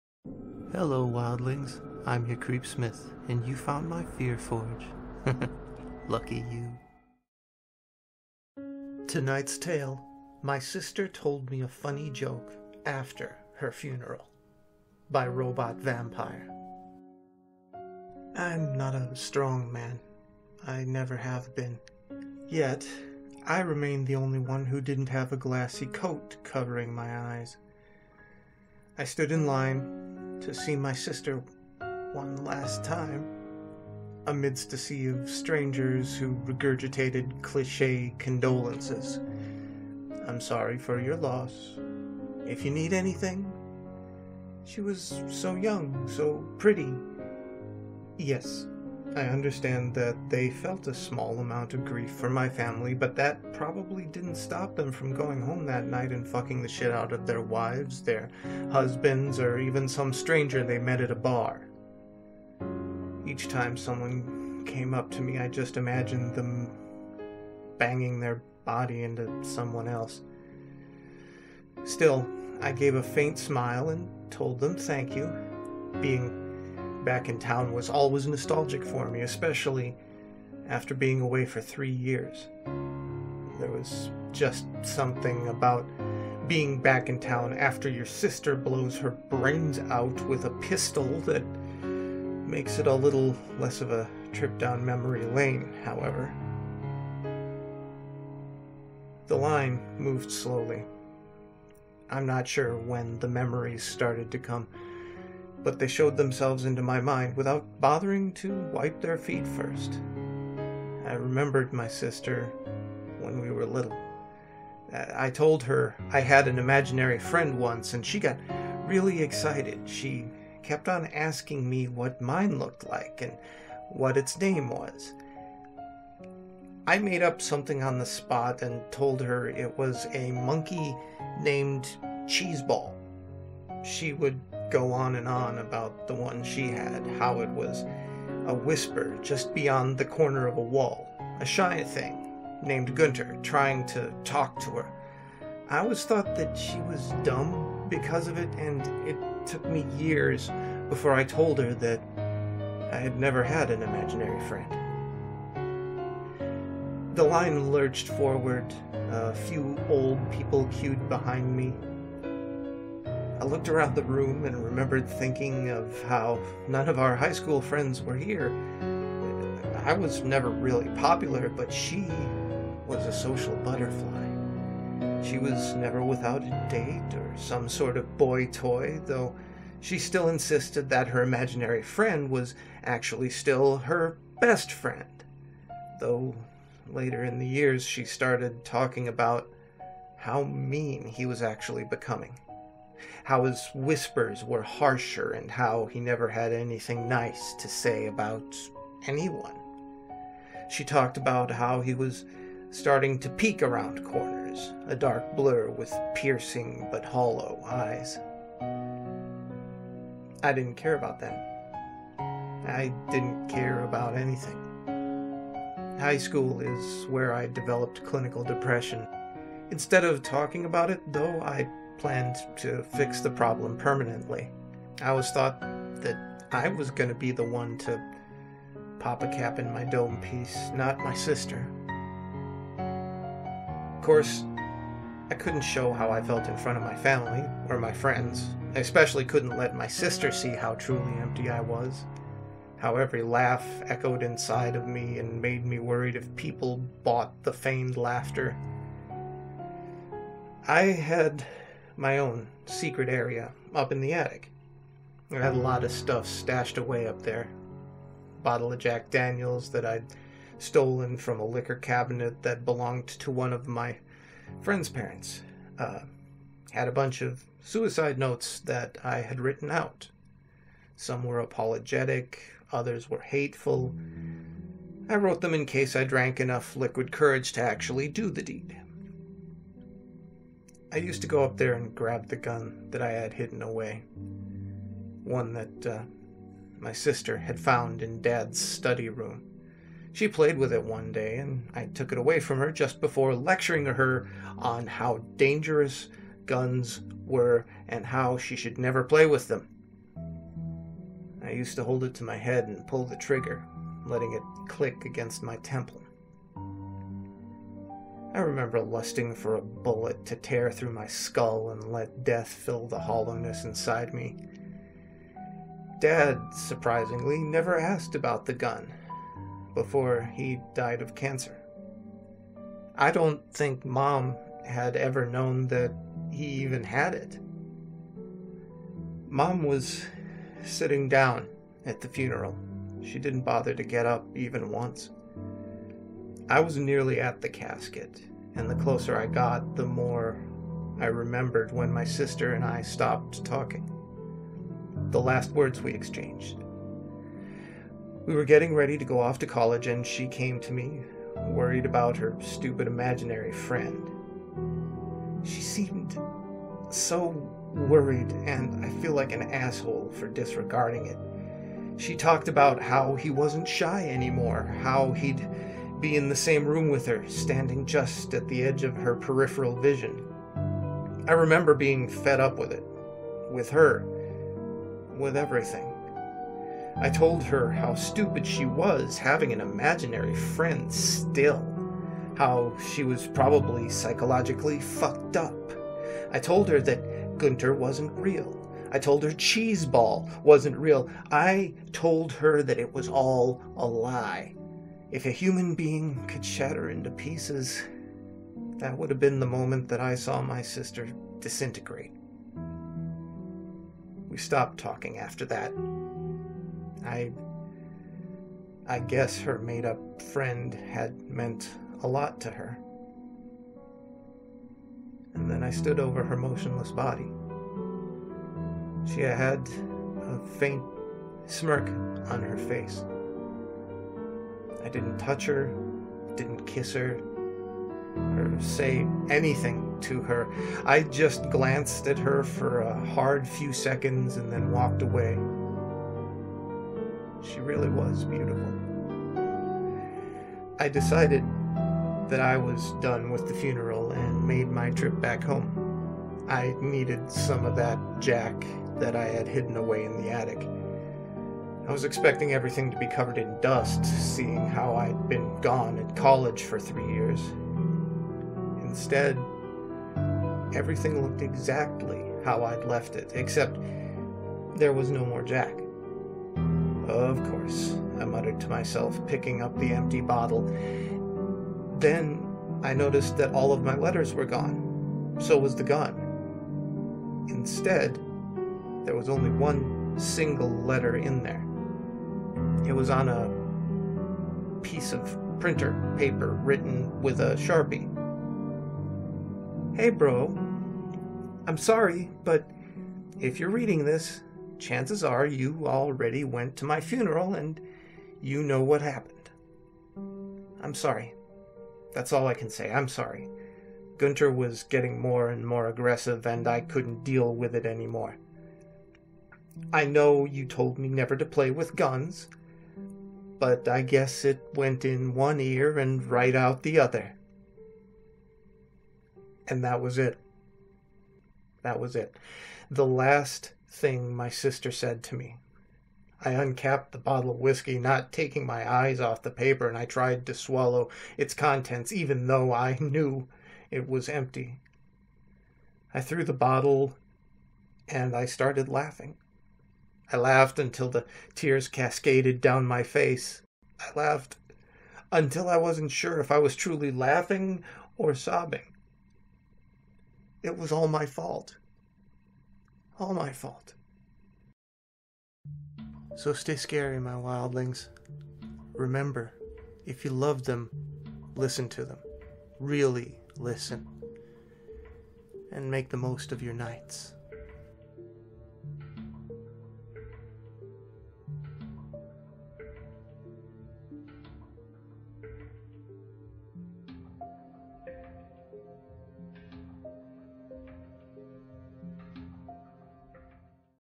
My Sister Told Me A Funny Joke After Her Funeral by Robot Vampire
It's rare that you see real tragedy coming. It's common, however, to blame yourself for not seeing it. I'd tell people not to do that, but people gonna do what they want...